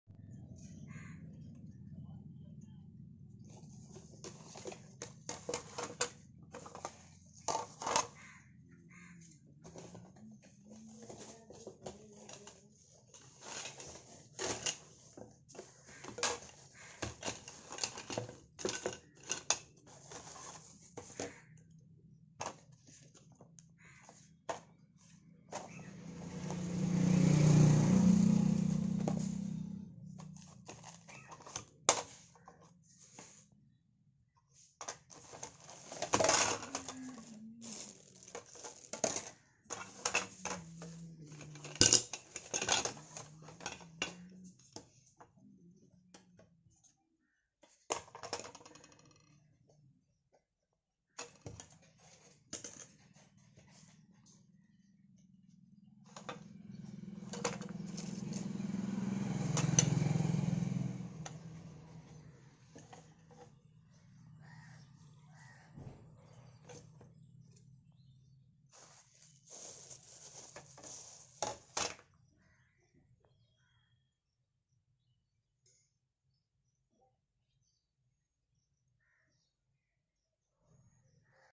Google pay business qr sticker stand and more earning with Google pay
Google pe me kaam Karne walo ko kya kya milta h company ki taraf se

Google pay stand qr review

Google pay se earning

 Google pay employees ko yahi sab stand qr milte Hain

and earnings tricks